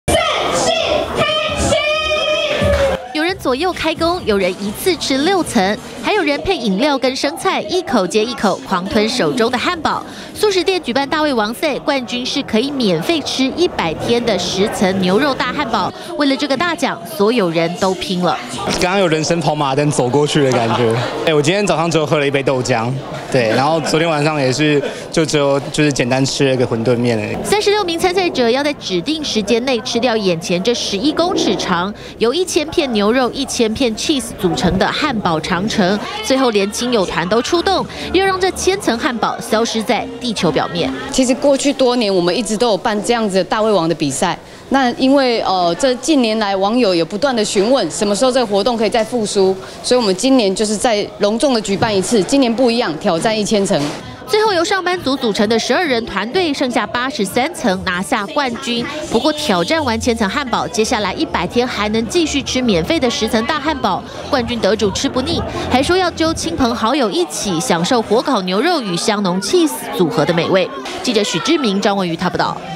20181116中天新聞 最狂漢堡長城！ 36人挑戰千層肉片漢堡
速食店舉辦大胃王賽，36名參賽者要在指定時間內，一起吃掉由千層肉片組成的漢堡長城，冠軍還可以免費吃一百天的十層大漢堡。